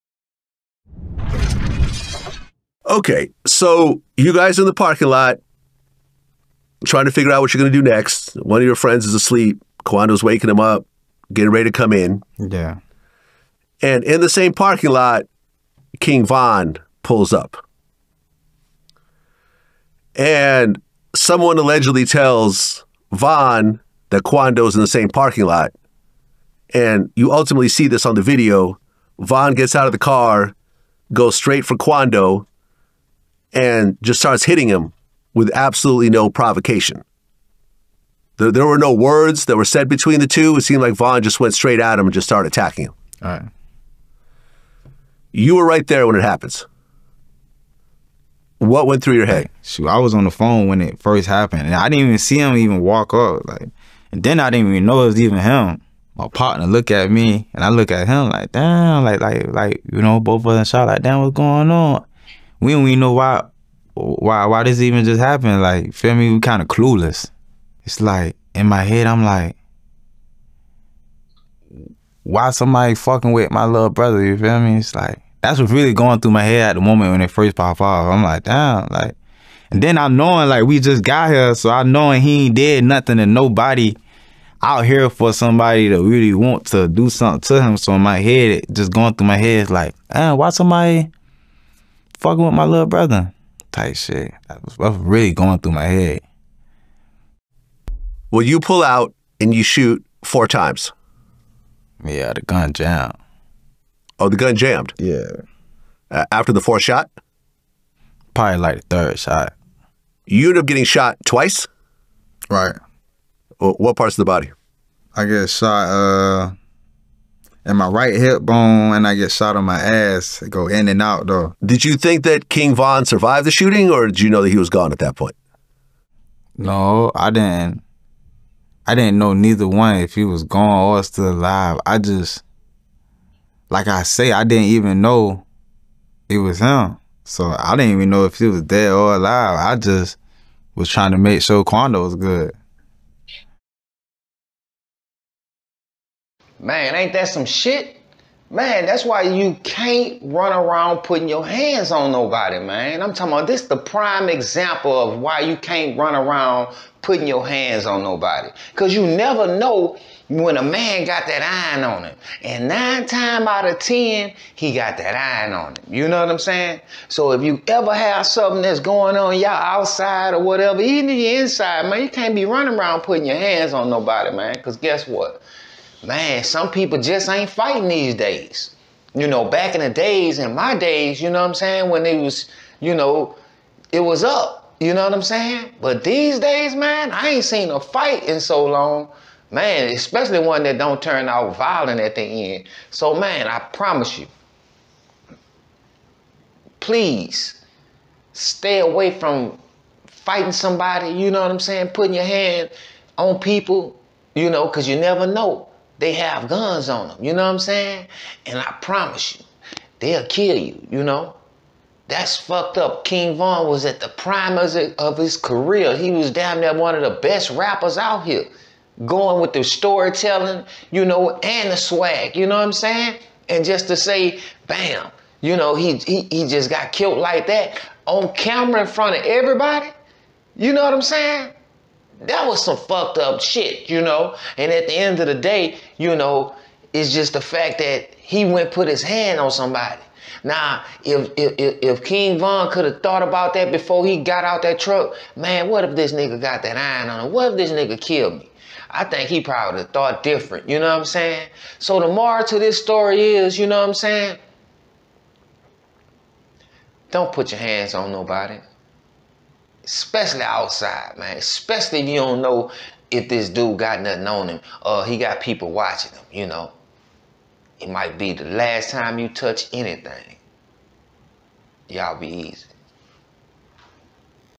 Lul Tim Speaks On Why He Shot King Von For Punching On Quando Rondo~ REACTION VIDEO
Facts Not Fiction podcast presents a reaction video of an interview. Creating and remixing videos, not out intentionally trying to steal anything from anybody.